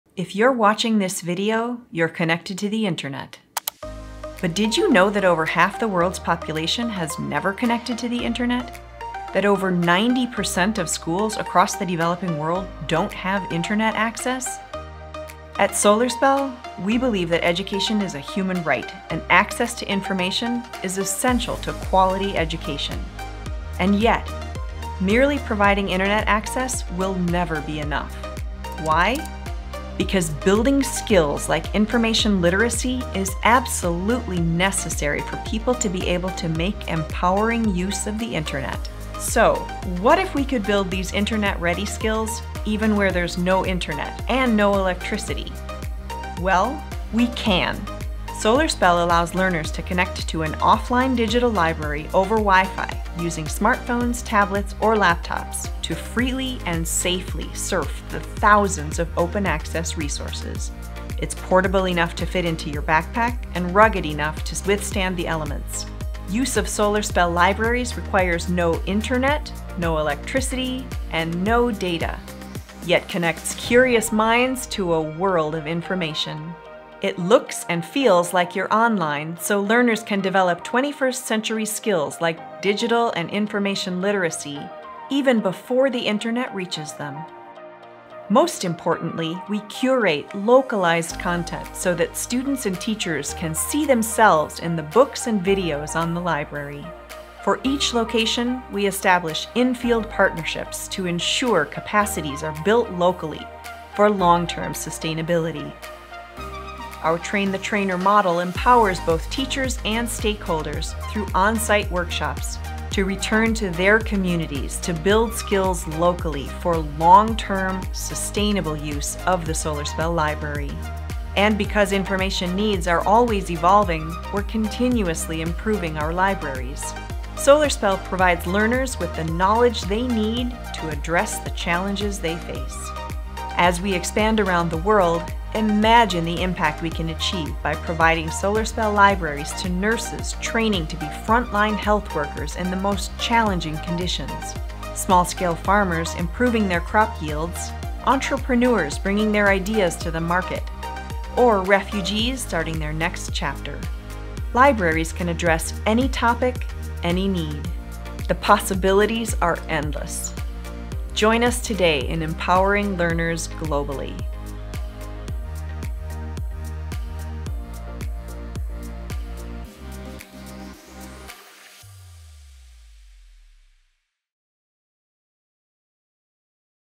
SolarSPELL offline digital libraries: Empowering learners globally
The ASU SolarSPELL Initiative empowers offline communities globally by providing localized digital libraries and building the internet-ready skills people need to make informed decisions, increase their self-reliance, and improve their quality of life.

We work closely with local partners to identify information needs, curate relevant content, and continually improve our collections across three sectors:  education, health and agriculture. The SolarSPELL (Solar Powered Educational Learning Library) digital library mimics an online experience by generating its own offline WiFi hotspot to which any WiFi-capable device can connect to freely, and safely, surf the library’s tens of thousands of open access resources.